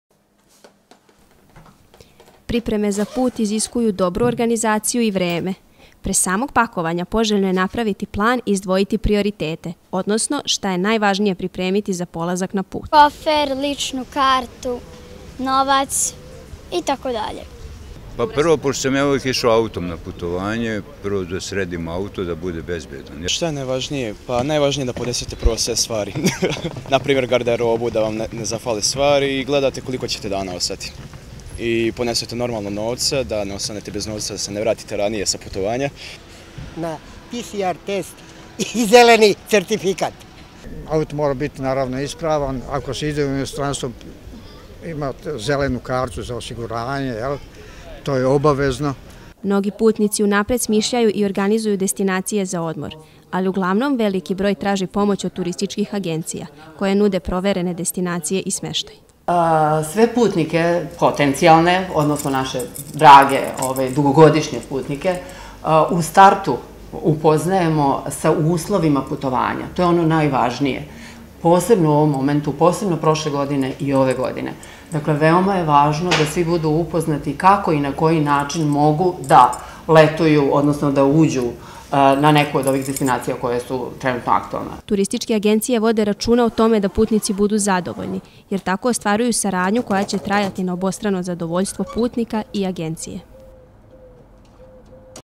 Putovanja preko turističke agencije
U vreme savremenih telefona, tableta, dostupnim internet stranicama, mnogo je lakse da sami pronadjemo i rezervisemo letovanje, ali i dalje se najcesce sve organizuje preko turistickih agencija, jer je tako bolje i sigurnije.